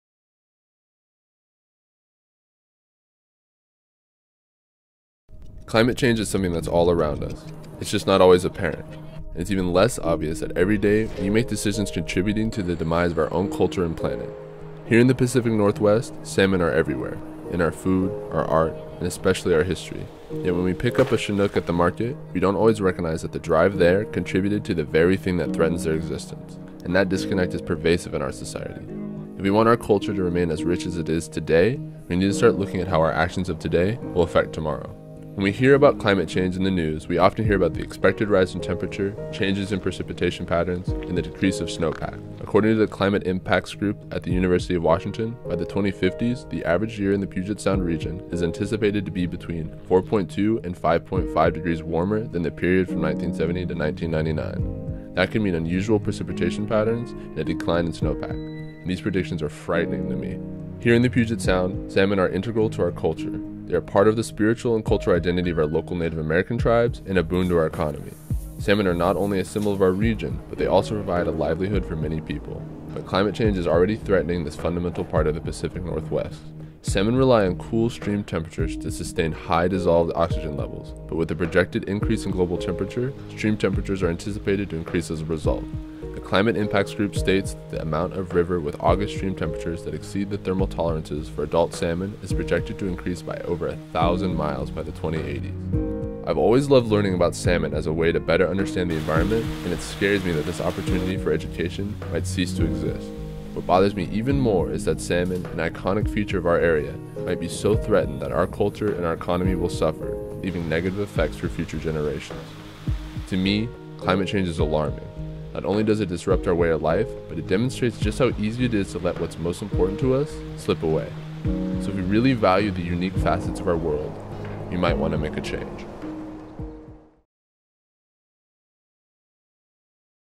Second Annual UW Climate Change Video Contest - College Videos, Second Place
The University of Washington School of Environmental and Forest Sciences (SEFS) launched the second annual UW Climate Change Video Contest in December 2015. SEFS asked Washington state high school and undergraduate college students to show the world what climate change meant to them in three minutes or less. The top 10 video submissions were screened at Town Hall on May 14, 2016 and the winners were announced by a lively panel of judges—Yoram Bauman, Dean Lisa J. Graumlich, Paul D. Miller (aka DJ Spooky) and Ethan Steinman. Cash prizes were awarded to the winning videos—$5,000 for first place; $1,000 for second place; and $500 for third place.

Second Place -   Ben Jensen, Charles Johnson, Anthony Whitfield - University of Washington

06/22/2016